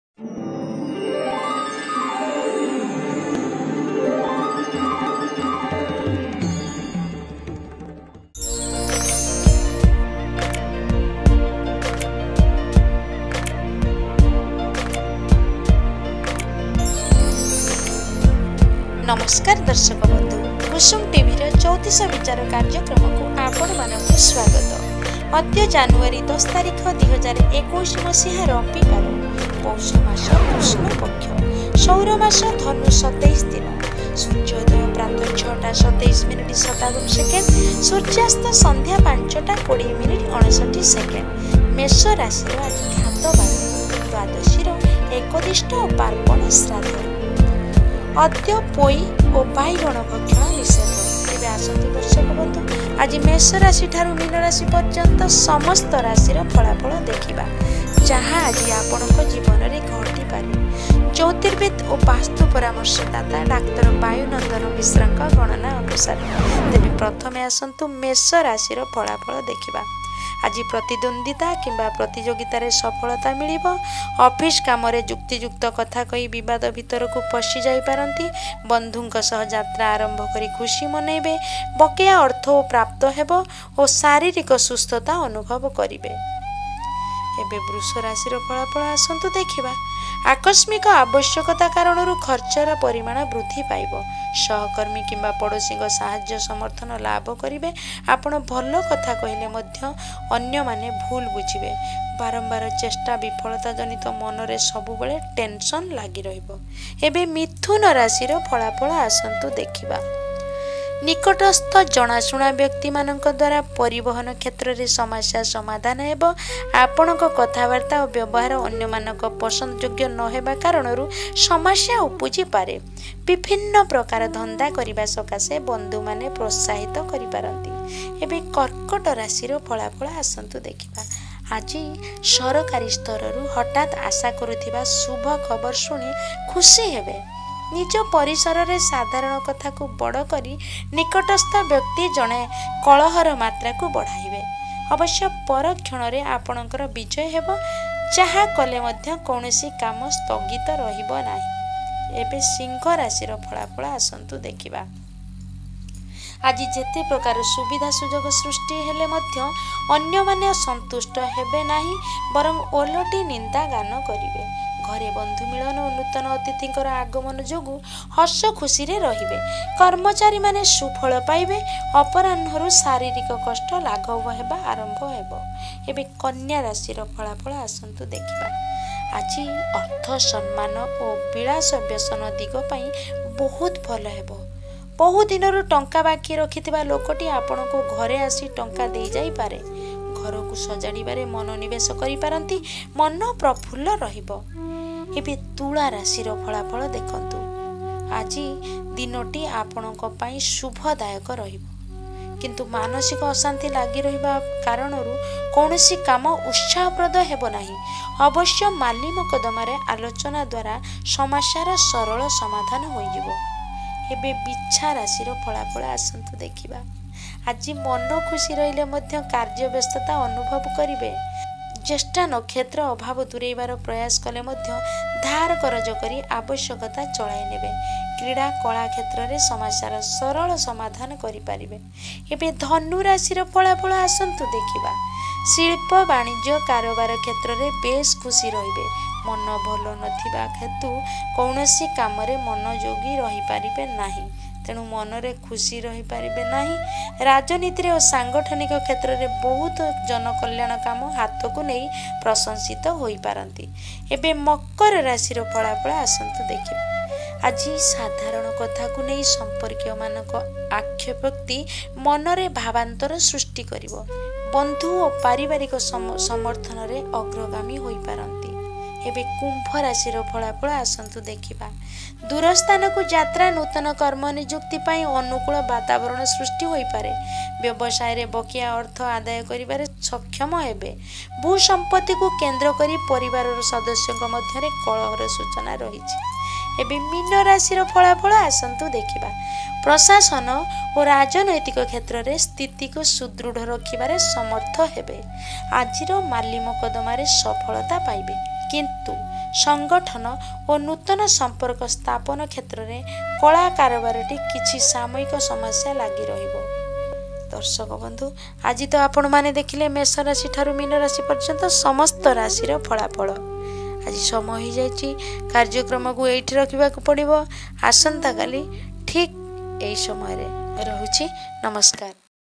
jyotish vichar10 01 2021
Jyotirvid & Vastu Bisarad
Dr Bayunandan Mishra nka ganana Anusare Aaji Ra Rasifala Yeha aapananka jivan re ghati pare.